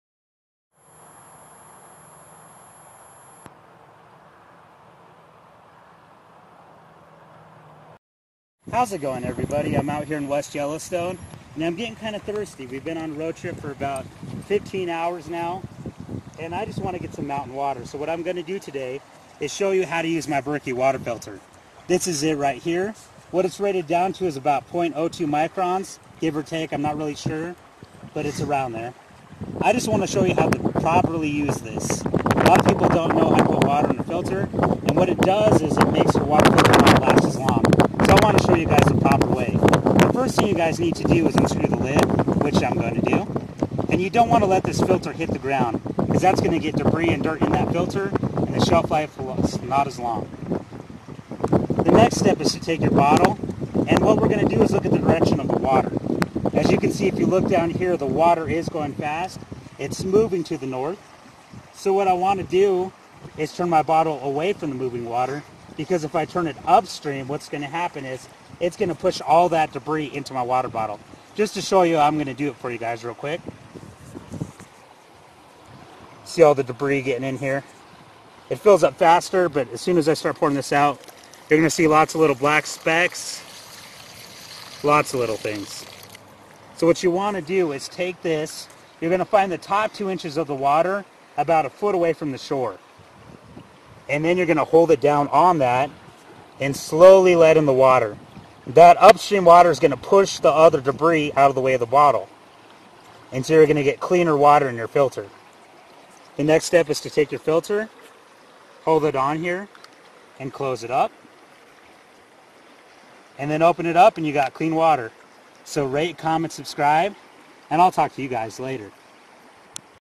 How to Use a Water Bottle Purifier (Sport Berky Demonstrated)
I show how to use a water bottle filter or purifier with my Berky Water Bottle.  This is located in West Yellowstone on the Gallatin River in Montana.